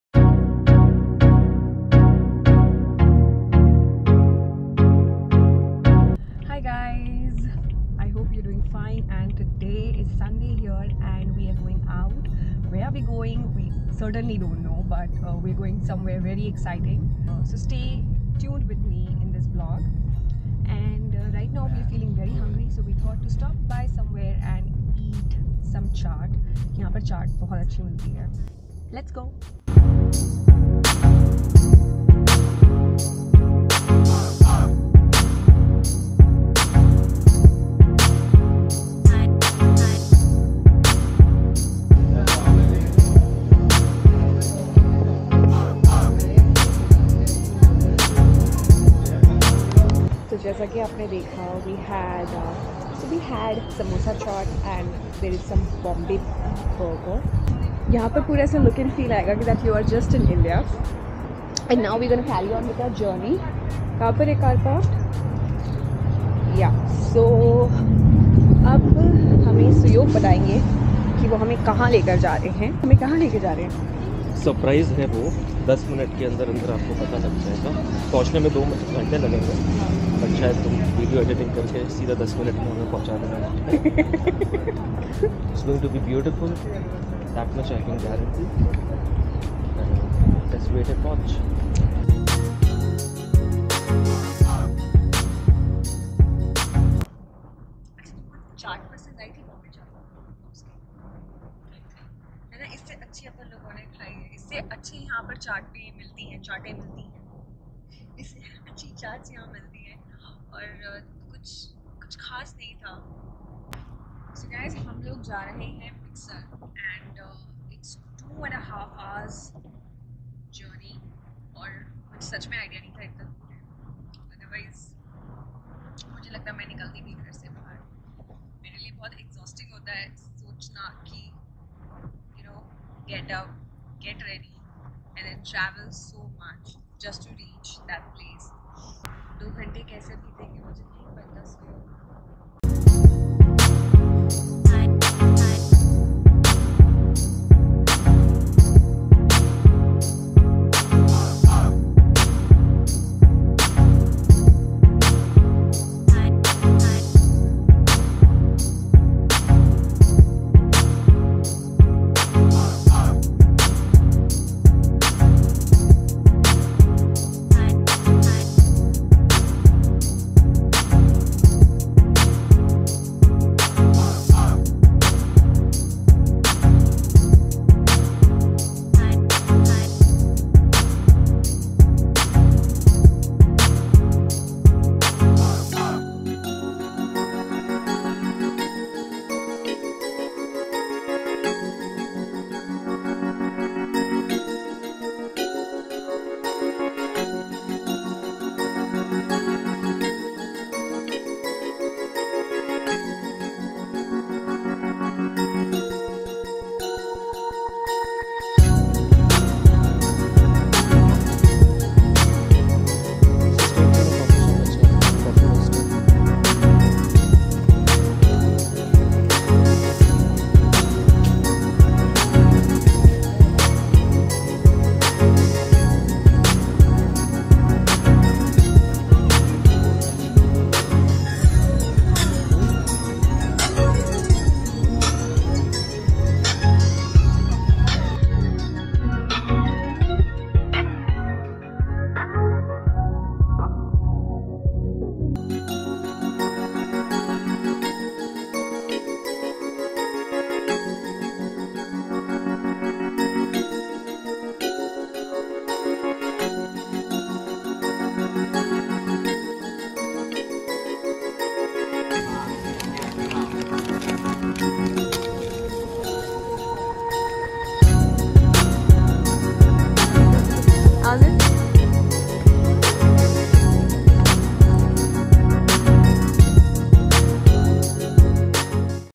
Sunday Suspense | Big Sur California | Carmel By The Sea Vlog
Here's a travel vlog of my Sunday. We went to Big Sur and Carmel by the Sea. Hope you enjoy watching it.